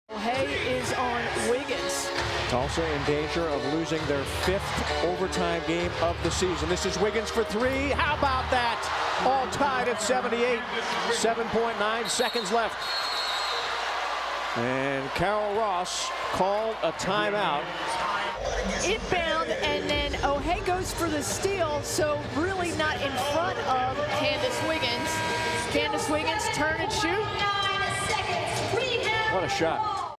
Candice Wiggins Ties it in OT with the Crazy 3 Pointer!
Check Out Candice Wiggins as she hits the crazy 3 pointer that would send the game into Double Overtime! Candice also ended the game with 20 points!